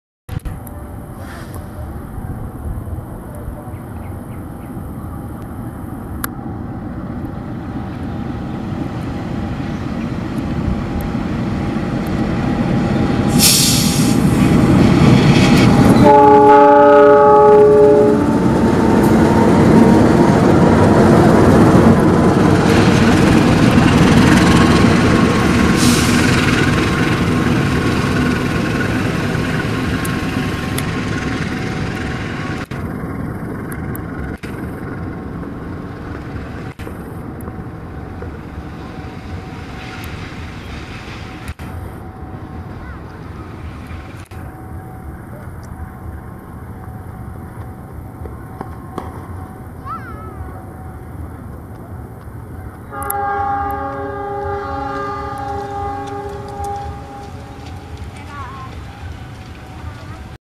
dua lokomotif yang bergandengan dengan konsep dirgahayu RI ke 76
lokasi : gunung Antang, Matraman Jakarta timur